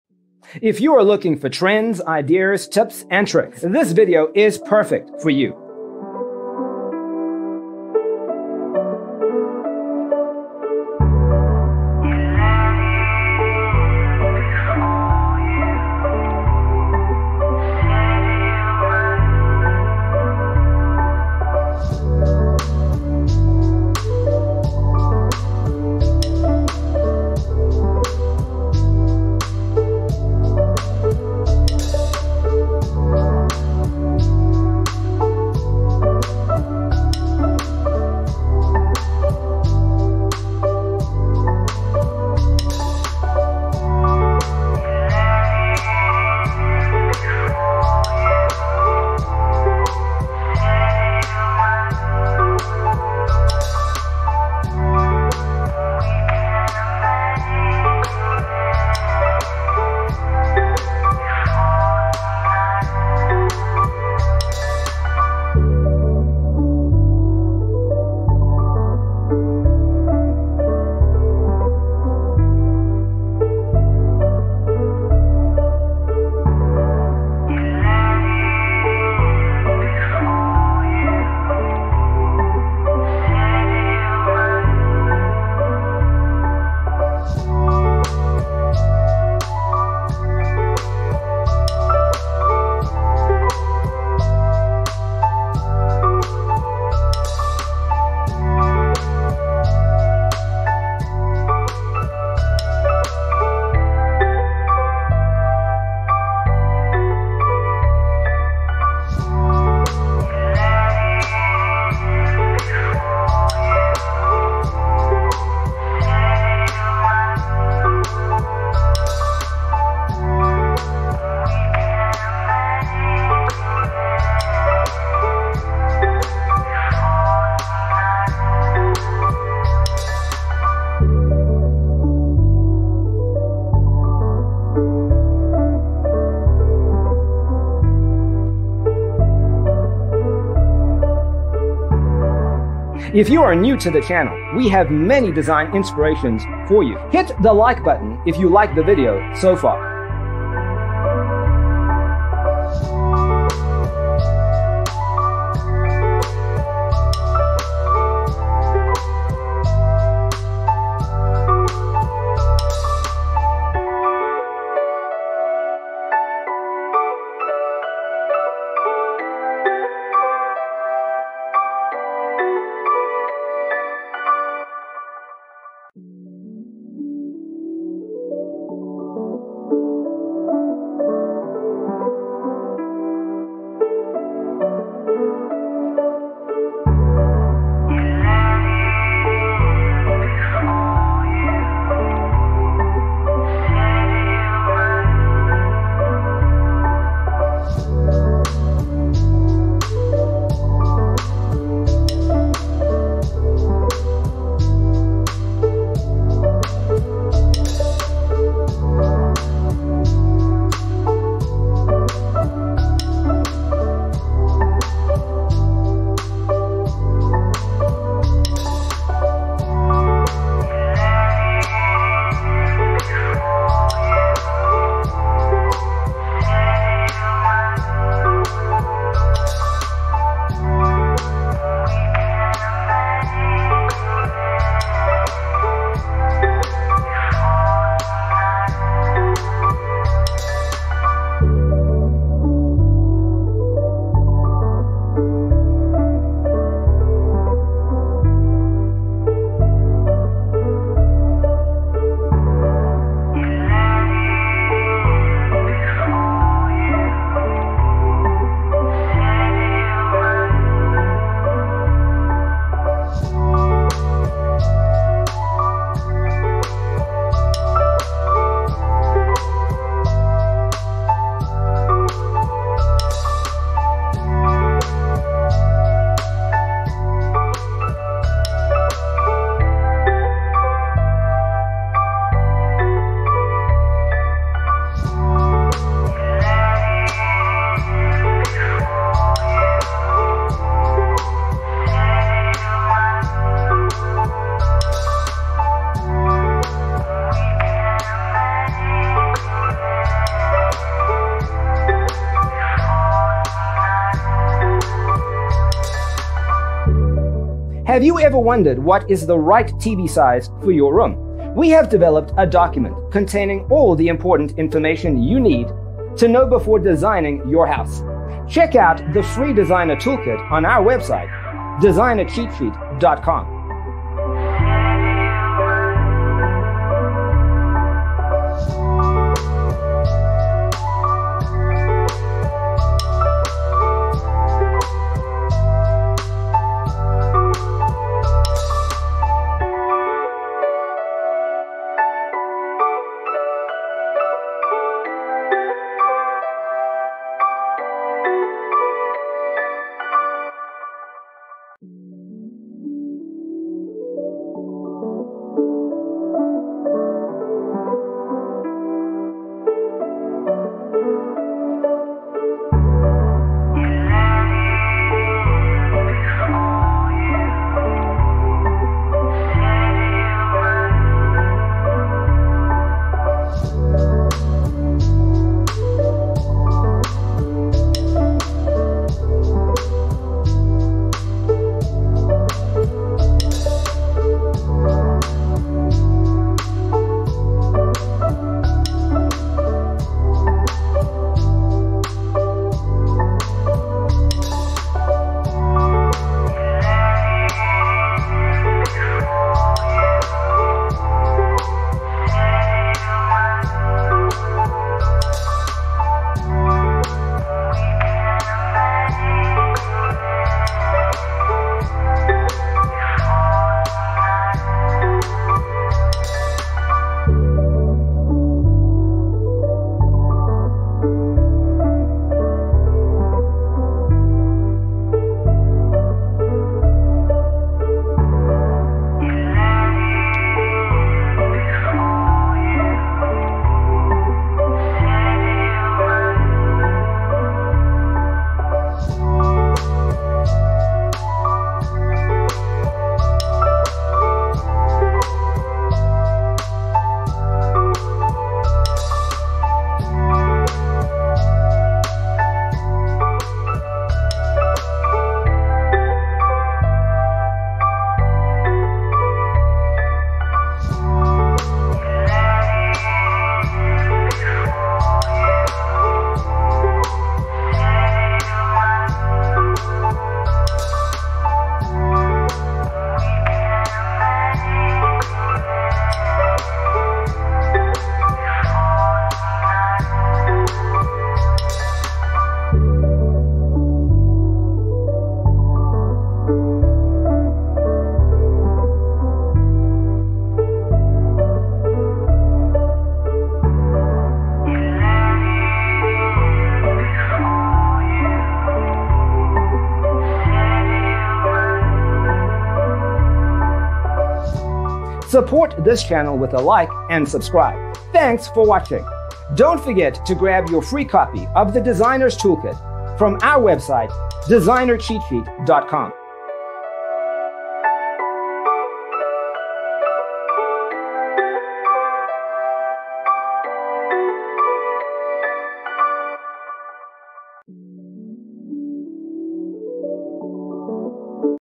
75 Loft-style Living Room With A Music Area Design Ideas You'll Love 🔴
0:00:00 - Intro
0:00:12 - Loft-style Living Room With A Music Area Design Ideas You'll Love: Inspirations 1 to 25
0:03:03 - Loft-style Living Room With A Music Area Design Ideas You'll Love: Inspirations 26 to 50
0:06:07 - Loft-style Living Room With A Music Area Design Ideas You'll Love: Inspirations 51 to 75